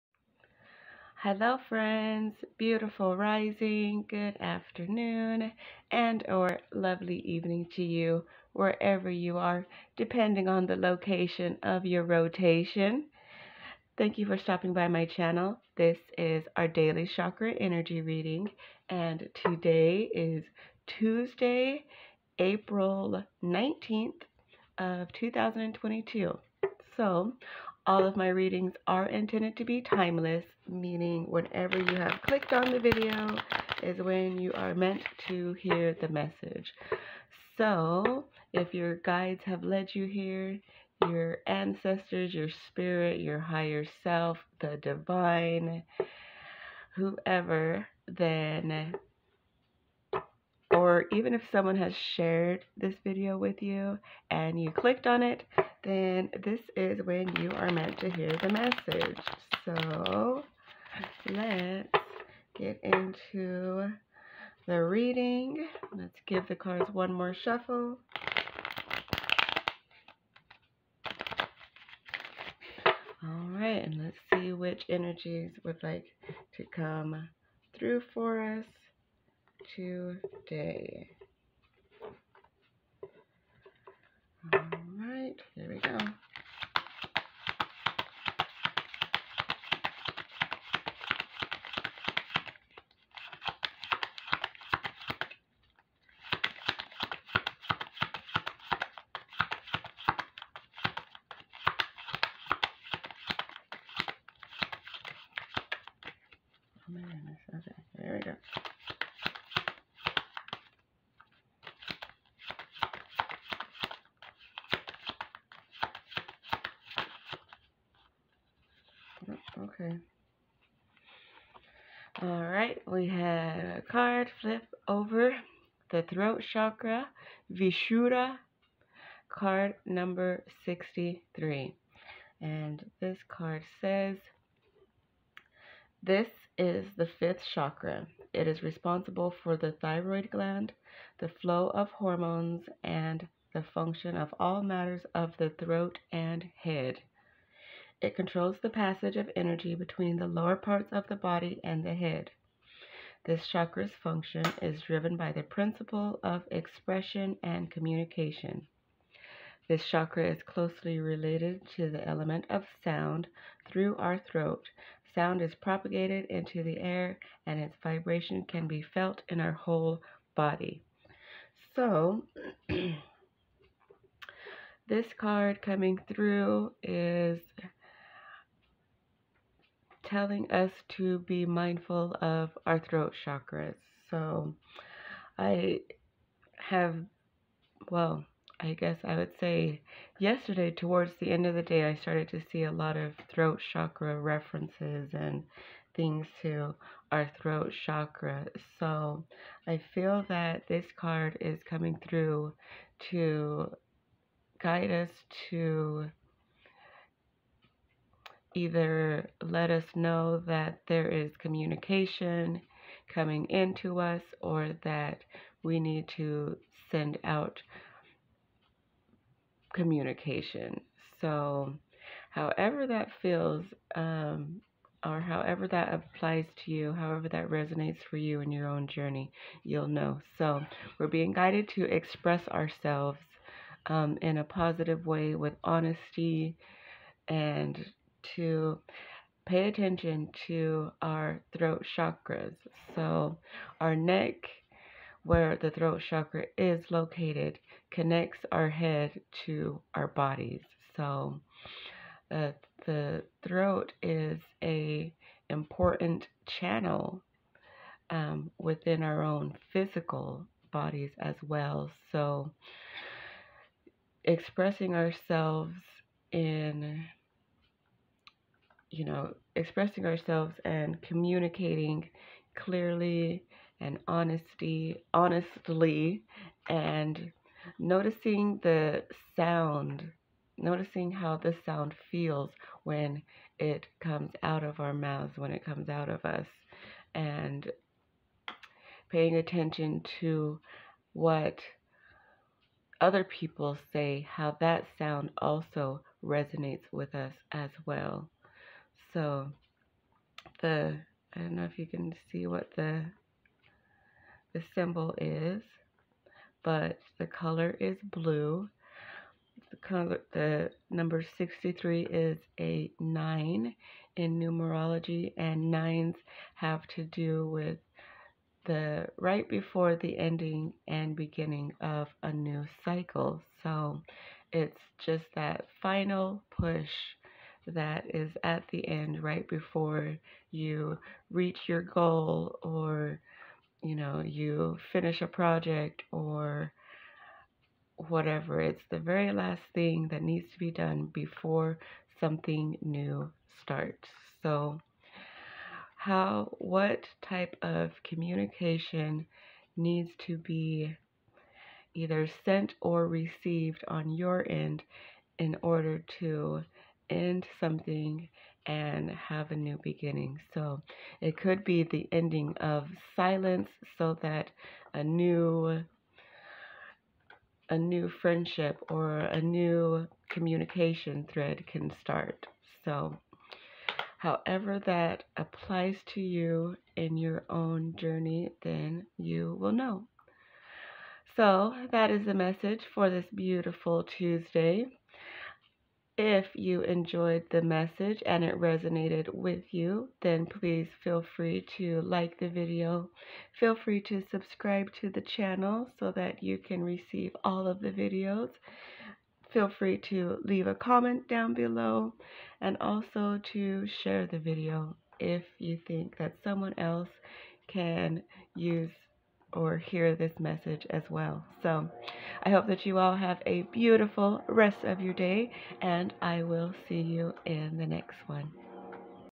2022 Daily Chakra energy - April 19
Happy April!!! 😆💐🌼🐇🐣

Welcome to my daily Chakra energy reading. These readings are timeless and intended to apply whenever you felt called to watch this message.

I will be pulling one card per day and reviewing the cards at the end of the week. So whenever you find this message, you are meant to hear it.

Thank you for coming on this journey with me. Let's grow together! 🥰🥰🥰

All Constitution rights invoked, none waived